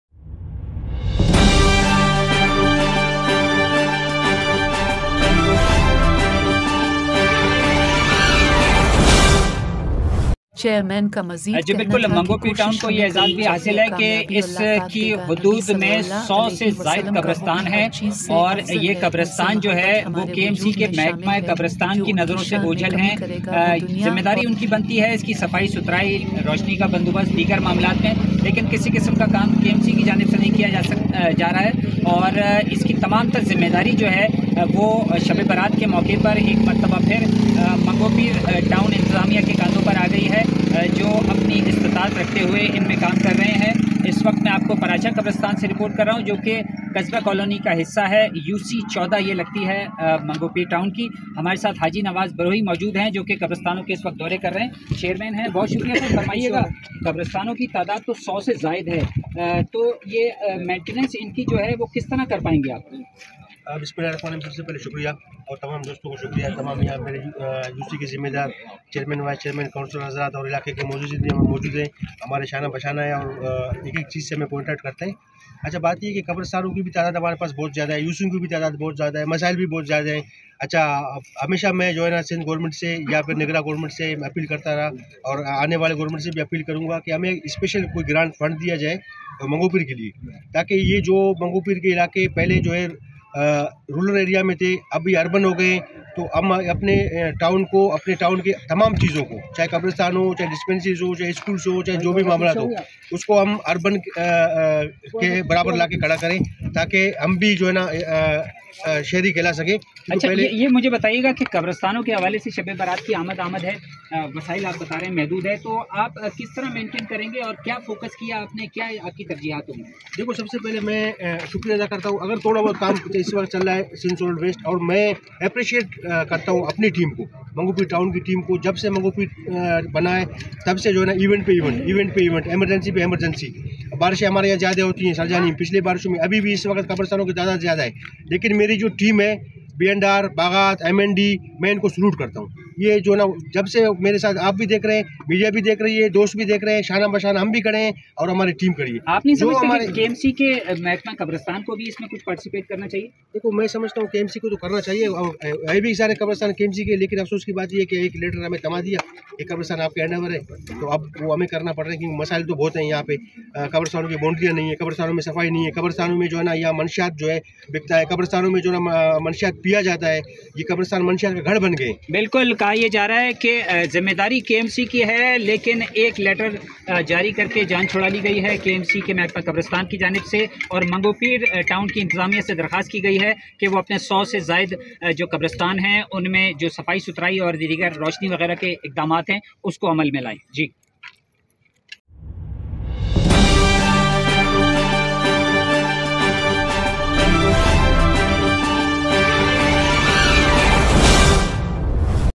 شب برات کے انتظامات کا جائزہ لینے کیلےچیئرمین منگوپیر ٹاؤن حاجی نواز بروہی نے قبرستانوں کا دورہ
شب برات کے انتظامات کا جائزہ لینے کیلےچیئرمین منگوپیر ٹاؤن حاجی نواز بروہی اپنی ٹیم کے ہمراہ قبرستانوں کا دورہ